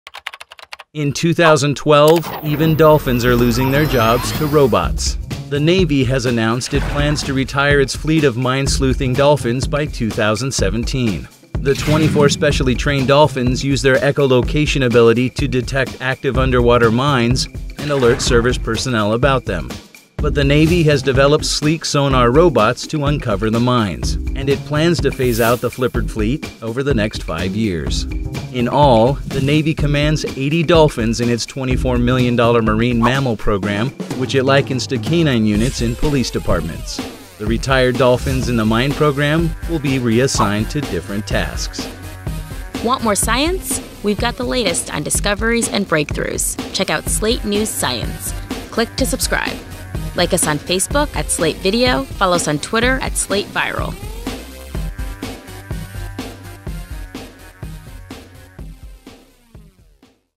Navy's Dolphin Work Outsourced to Robots
In 2012, even dolphins are losing their jobs to robots.

The Navy has announced it plans to retire its fleet of mine-sleuthing dolphins by 2017. The 24 specially trained dolphins use their echolocation ability to detect active underwater mines and alert service personnel about them.

But the Navy has developed sleek sonar robots to uncover the mines, and it plans to phase out the flippered fleet over the next five years.

In all, the Navy commands 80 dolphins in its $24 million marine mammal program, which it likens to canine units in police departments. The retired dolphins in the mine program will be reassigned to different tasks.